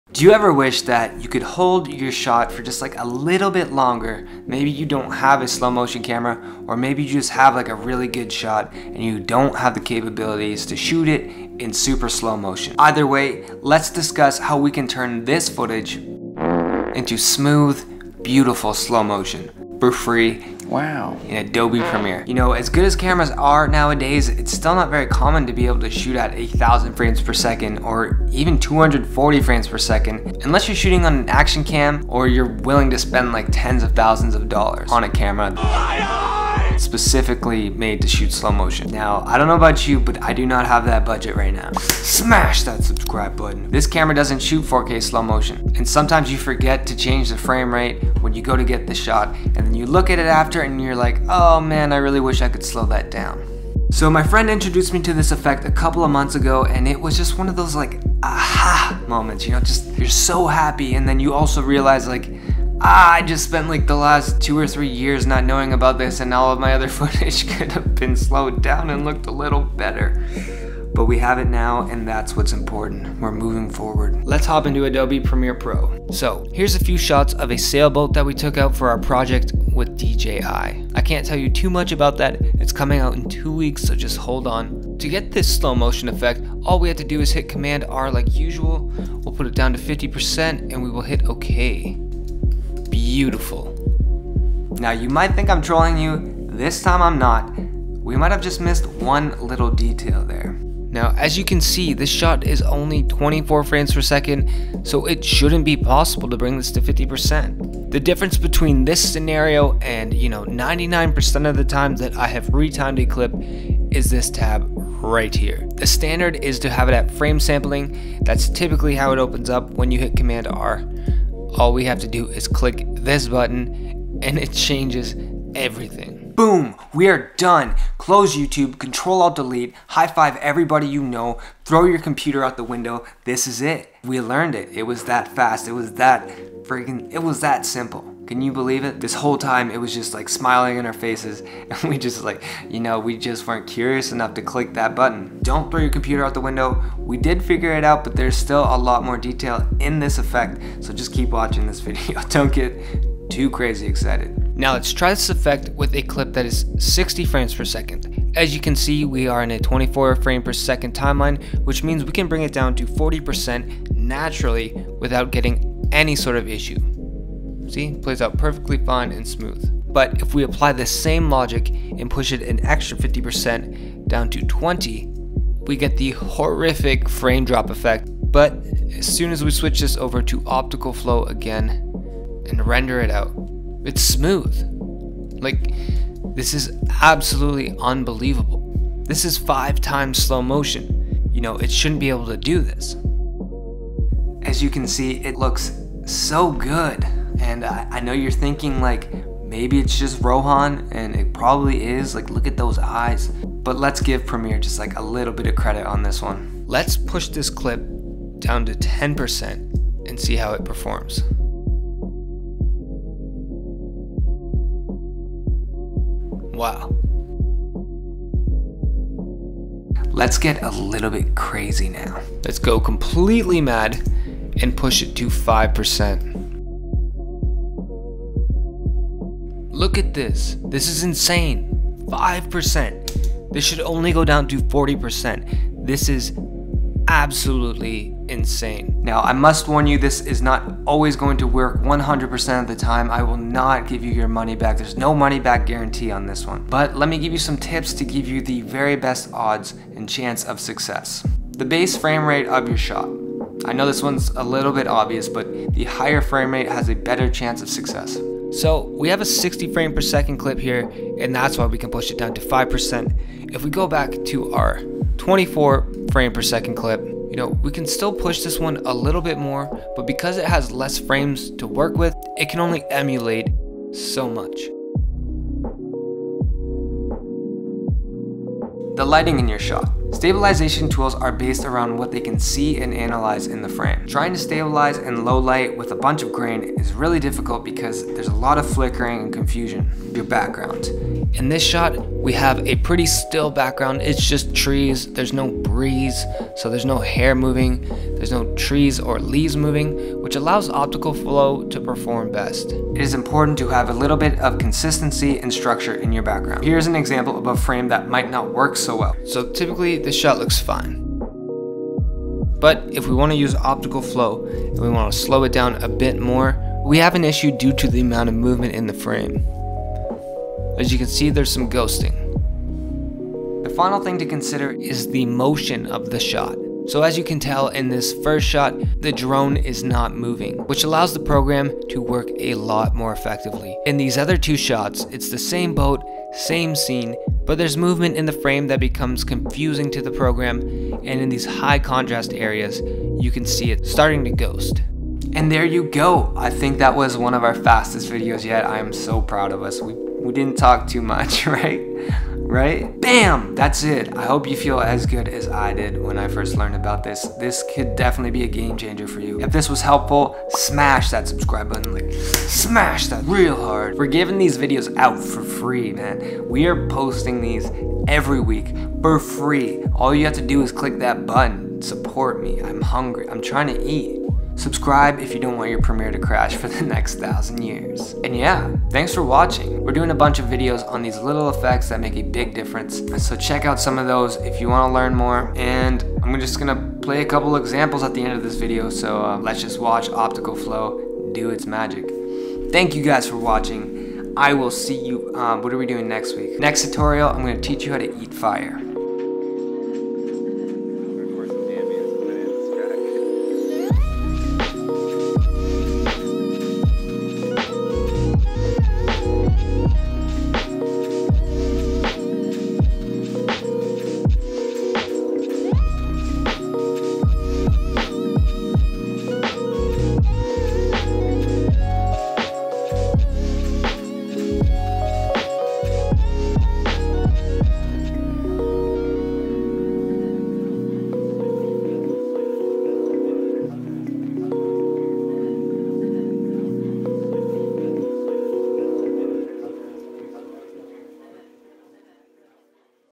SUPER SMOOTH SLOW MOTION VIDEO | PREMIERE PRO | NO PLUG-INS
Want better slomo but don't have a Slow Motion Video Camera?! NO PROBLEM! You can still make SUPER SMOOTH SLOW MOTION VIDEOS Using Adobe Premiere Pro - NO PLUG-INS REQUIRED!

Turn 24 FPS into 240 FPS!! Perfect for drone shots!

Do you ever wish that you could hold a shot for that LITTLE bit longer, but don’t have a slow motion camera that shoots 1000 frames per second or even 240 frames per second? Most cameras can barely shoot 1080p in 120fps or 4K in 60fps and Slow Motion cameras are insanely expensive.

Thankfully, Adobe Premiere Pro has our back!! Their built-in software has come a very long way, and allowed us to create smooth slow motion without any plug-ins!

I just learned about this effect in Premiere Pro a couple of months ago and it was one of those “aha” moments. This is definitely a game-changer!